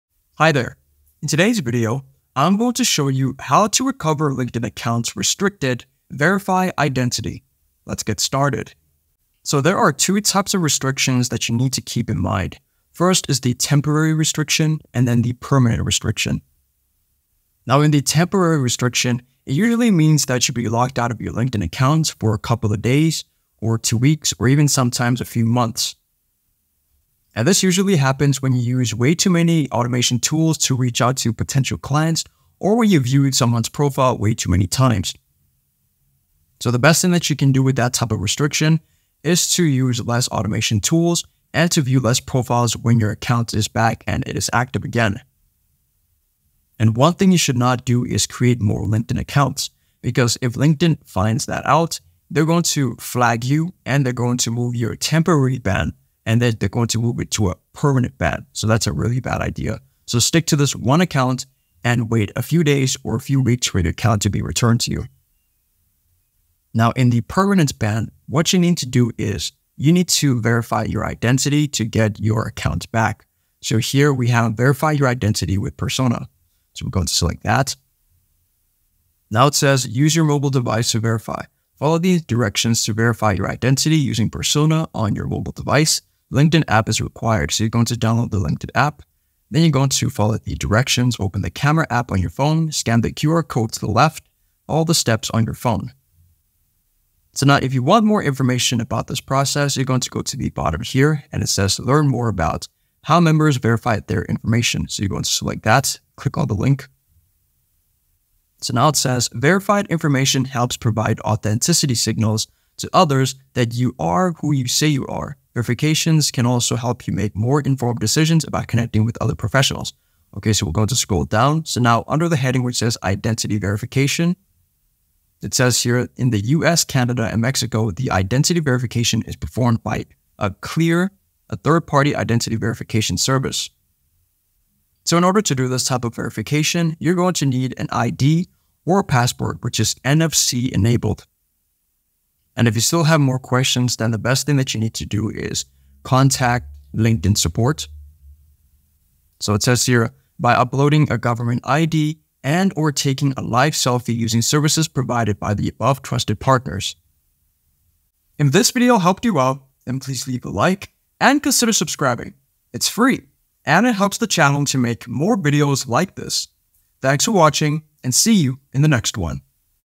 How To Recover LinkedIn Account Restricted Verify Identity (2024)
How To Recover LinkedIn Account Restricted Verify Identity

In this LinkedIn Recovery guide tutorial video I'll show you how to recover restricted linkedin account. Are you wondering what to do if linkedin restricted your account? Don't look further because after watching this video you'll be able to get back your restricted linkdedin account.

👍🏻 Did you get any value out of this video ?